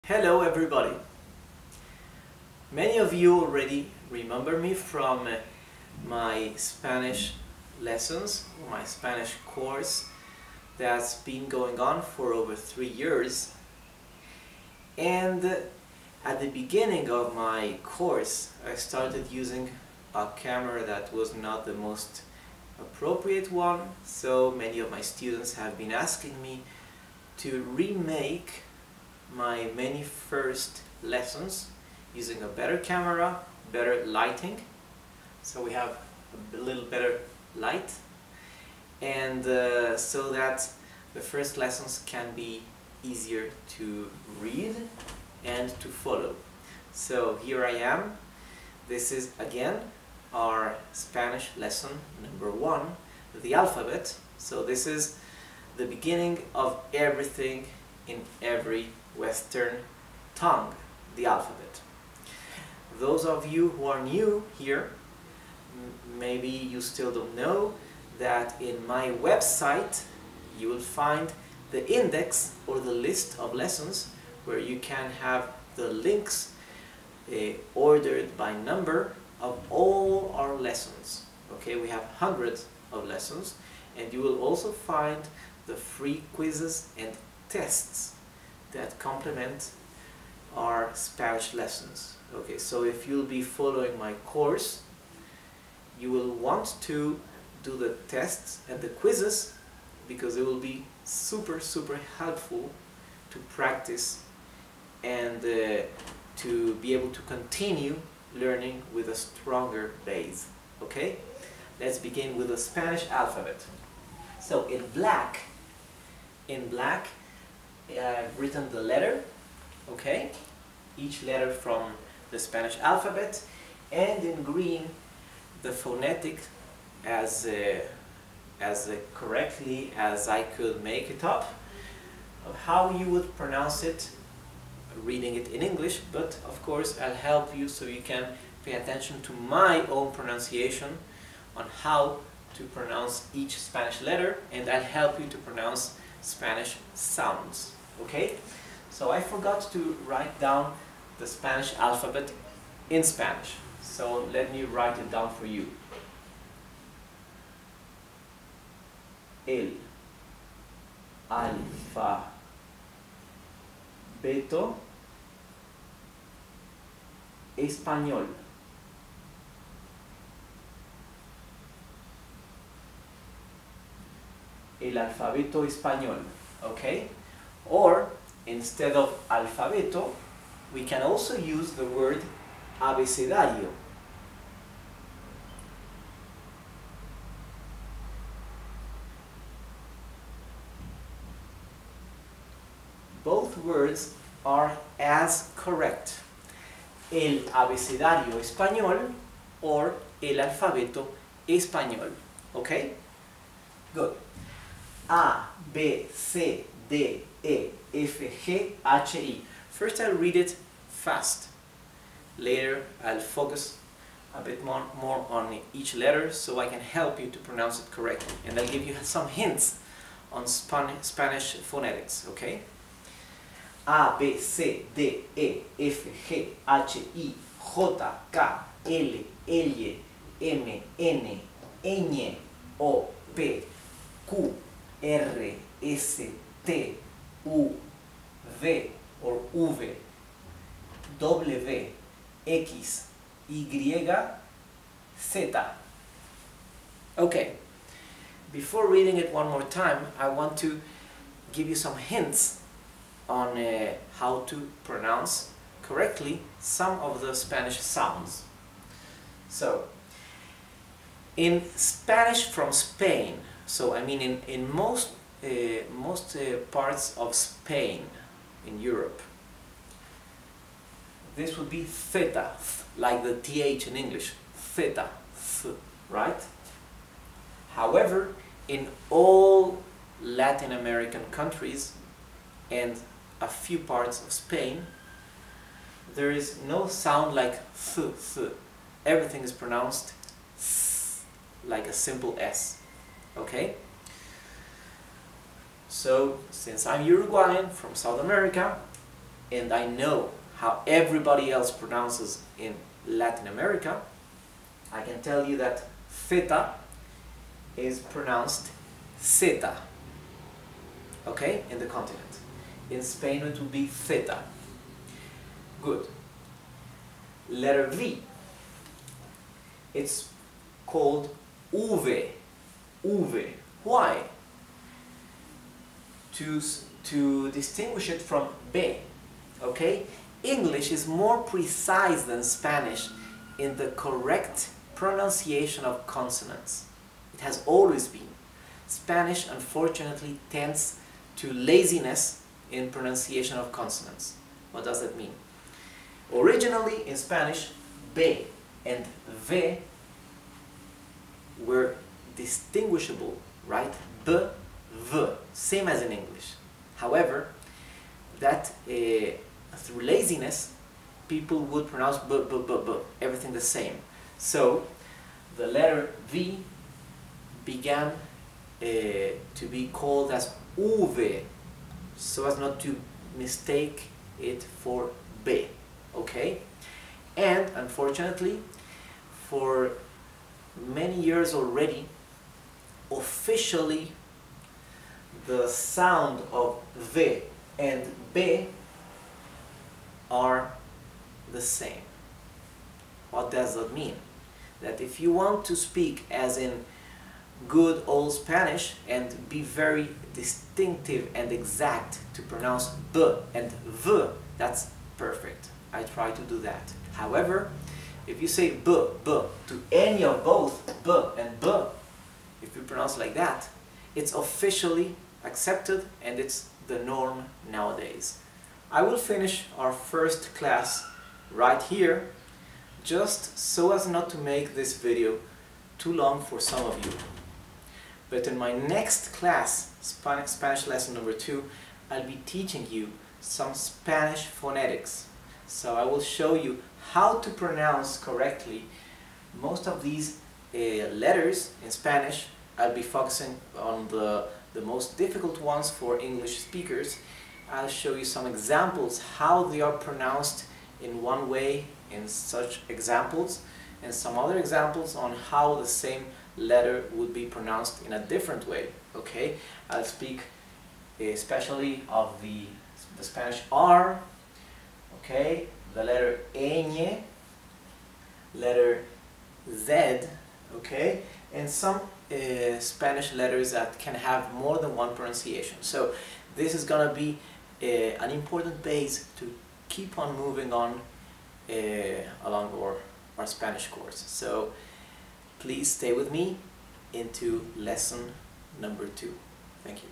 Spanish Alphabet + Spanish Pronunciation LEARN SPANISH 1
Spanish alphabet & Spanish pronunciation for  English speakers. This Spanish Class 1 is a remake of the older version of Spanish lesson 1 from 2013: The very first thing you need to know to learn Spanish fast is the Spanish alphabet and the right Spanish pronunciation, with its variations. I will be sharing with you some few insights on phonetics, linguistics and Spanish culture.